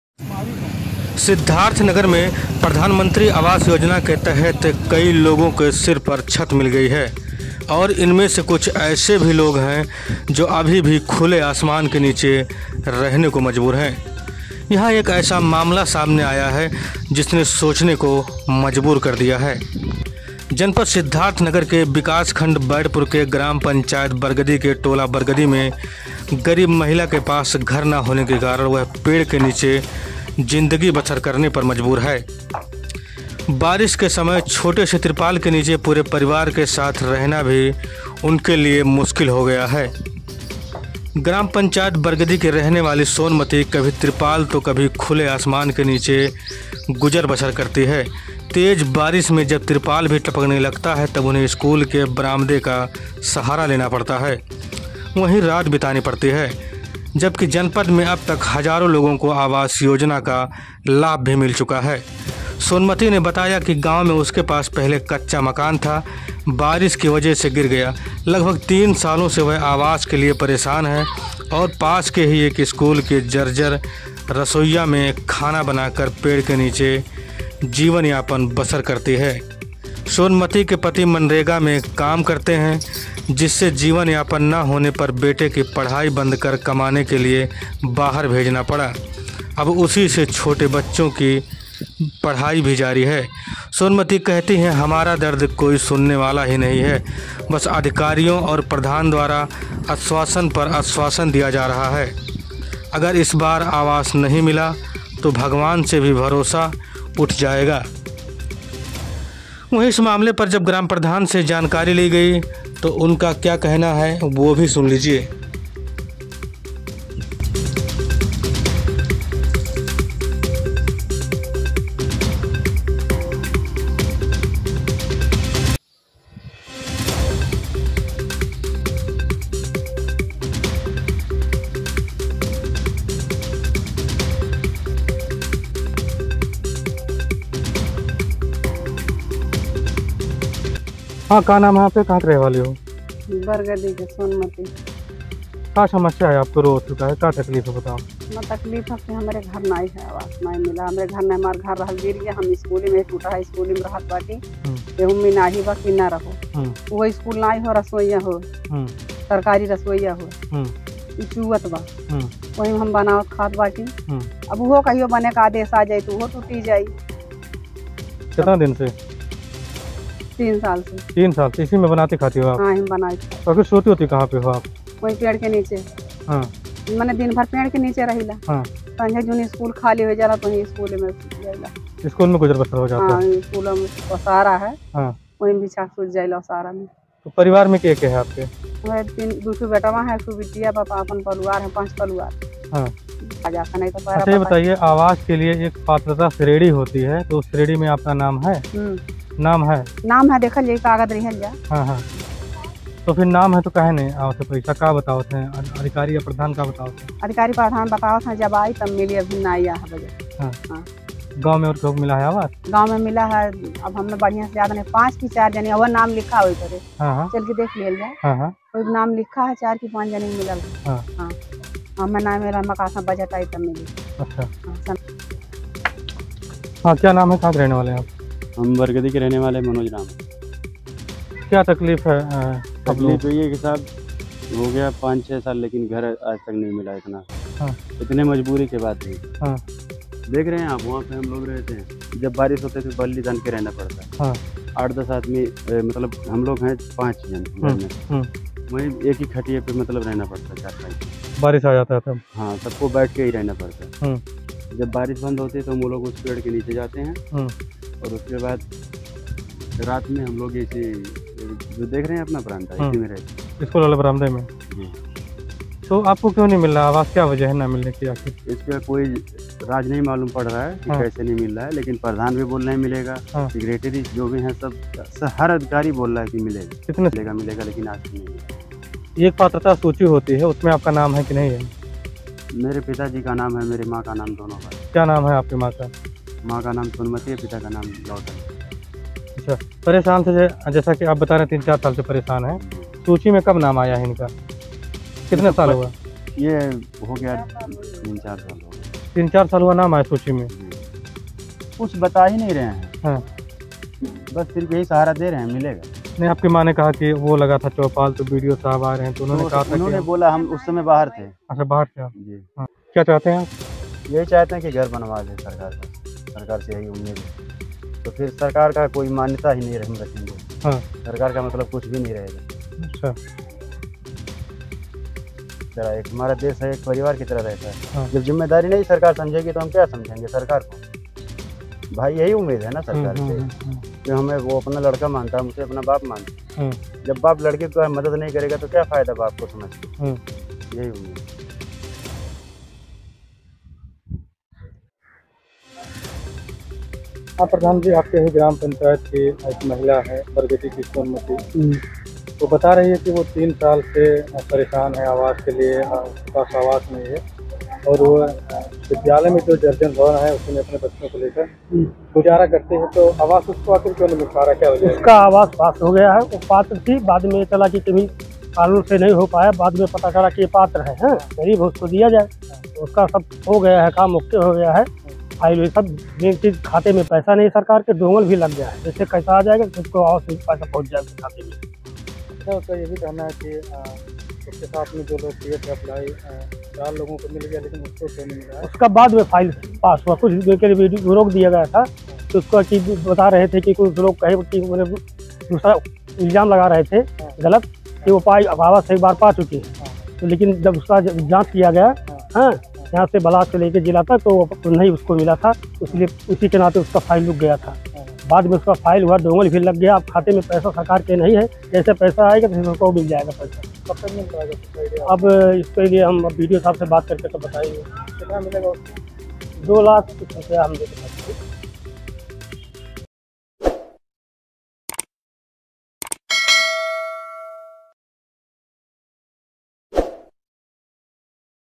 महिला ने रोकी रिपोर्टर की गाडी || फिर जो हुआ देखकर हो जाओगे हैरान
प्रधानमंत्री आवास योजना के तहत कई लोगों को सिर पर छत मिल गई है और इनमें से कुछ ऐसे लोग हैं जो अभी भी खुले आसमान के नीचे रहने को मजबूर हैं ।
 जनपद सिद्धार्थनगर के विकासखंड बर्डपुर के ग्राम पंचायत बरगदी के टोला बरगदी में गरीब महिला के पास घर न होने के कारण वह पेड़ के नीचे जिंदगी बसर करने पर मजबूर है । बारिश के समय छोटे से त्रिपाल के नीचे पूरे परिवार के साथ रहना भी उनके लिए मुश्किल हो गया है  ।ग्राम पंचायत बरगदी की रहने वाली सोनमती पेड़ के नीचे  गुजर बसर करती हैं । सोनमती ने बताया कि गांव में उसके पास पहले कच्चा मकान था बारिश के वजह से गिर गया लगभग 3 सालों से वह आवास के लिए परेशान है और पास के ही एक स्कूल के जर्जर रसोईया में खाना बनाकर पेड़ के नीचे जीवन यापन बसर करती है ।